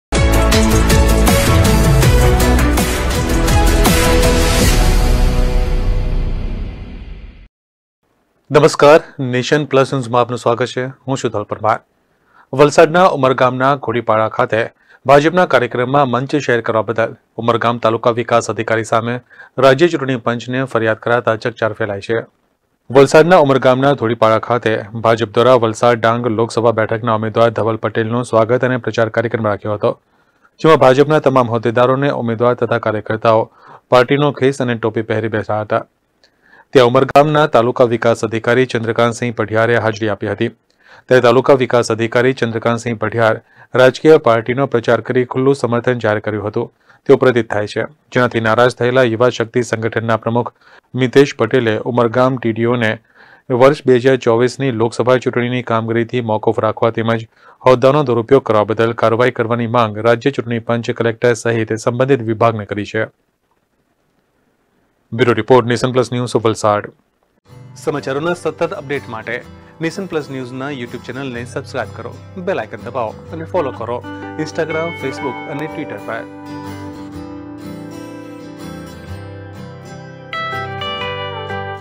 TDO ભાજપના ઉમેદવારના કાર્યક્રમમાં સ્ટેજ પર !
વલસાડના ઉમરગામના ધોડીપાડા ખાતે ભાજપના કાર્યક્રમમાં મંચ શેર કરવા બદલ ઉમરગામ તાલુકા વિકાસ અધિકારી સામે રાજ્ય ચૂંટણી પંચને ફરિયાદ કરાતા ચકચાર ફેલાઇ છે.